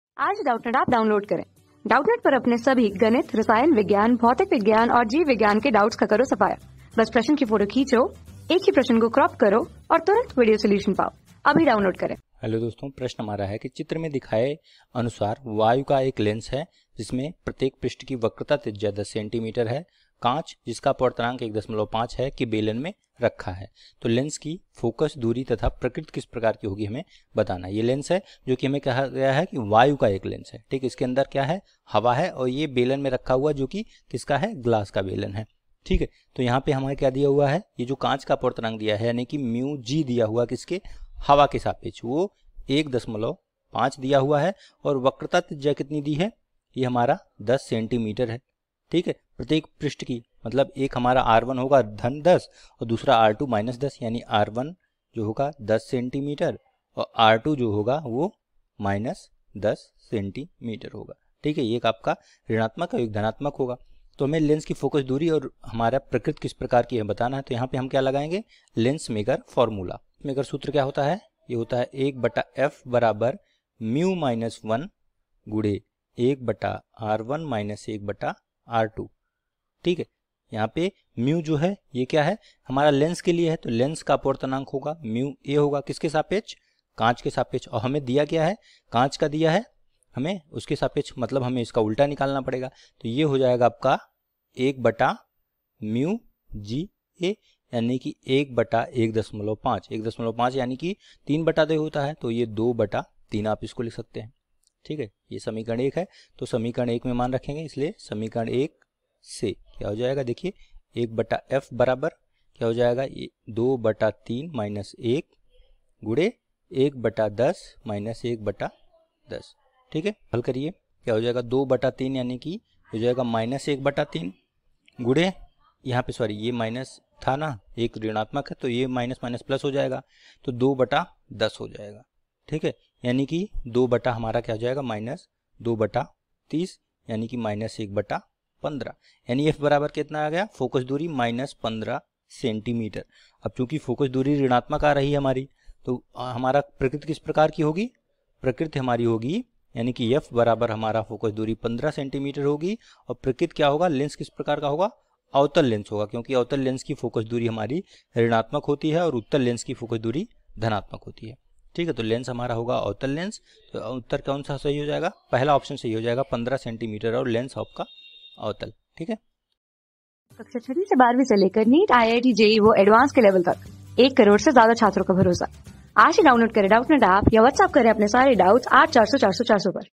चित्र में दर्शाये अनुसारवायु का एक लेंसजिसकेप्रत्येकपृष्ठ की वक्रता त्रिज्या 10 सेमी है काँच (mu...
चित्र में दर्शाये अनुसारवायु का एक लेंसजिसकेप्रत्येकपृष्ठ की वक्रता त्रिज्या 10 सेमी है काँच (mu =1.5)के बेलन में है।लेंस कीफोकसदूरीतथाप्रकृतिहोगी।
Class: 12
Subject: PHYSICS
Chapter: गोलीय पृष्ठ  से अपवर्तन
Board:Madhya Pradesh Board

👉 Have Any Query? Ask Us.